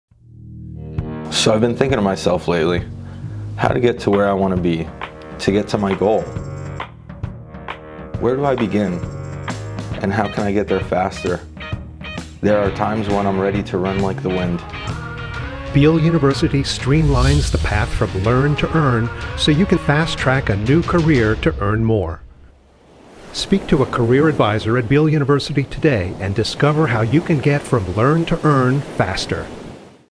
Time is valuable!
Many people forego a university degree because the traditional college path takes too long or doesn’t fit into their lives. Beal offers 28 accelerated degree programs across 6 fields of study that allow you to begin quickly, graduate sooner and reach your aim faster.